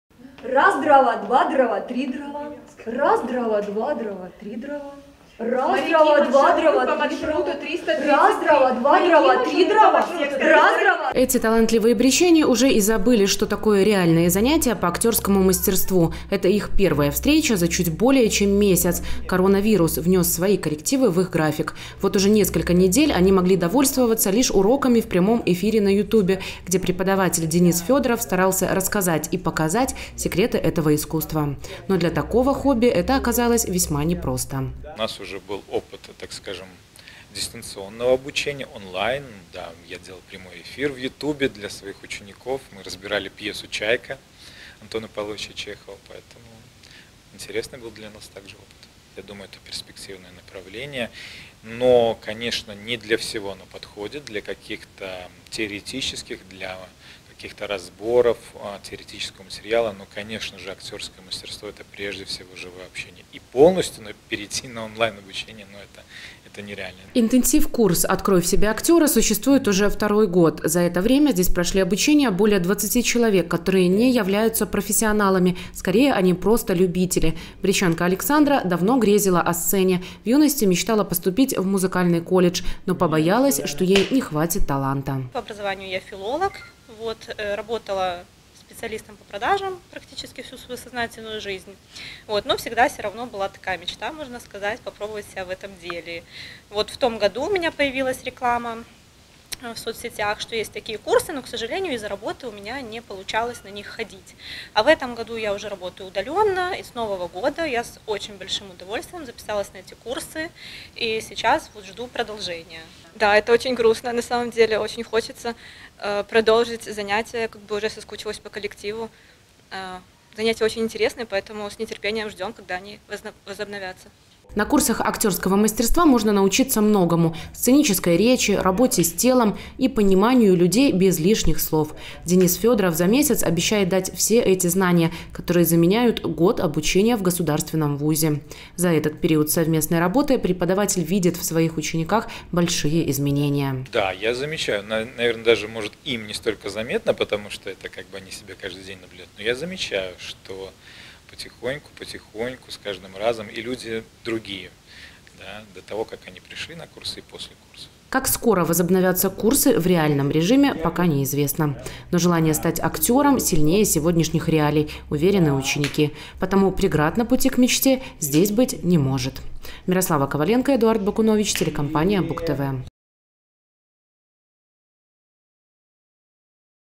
2020-05-01 г. Брест. Дистанционное обучение на курсах актеров. Новости на Буг-ТВ.
Журналист М. Коваленко. Оператор Э. Бакунович
Настоящий артист – всегда артист, даже на сцене – сказал когда-то журналист Михаил Генин. В сегодняшних реалиях можно смело перефразировать эту цитату. Настоящий артист – всегда артист, даже в период пандемии. Талантливые брестчане, слушатели курсов актёрского мастерства, ни дня не могут прожить без этого творческого хобби. Потому нашли выход – обучение посредством прямых эфиров. Но не забыли и про реальные занятия. На одном из таких побывала Мирослава Коваленко и узнала, что настоящему актёру ничего не помешает.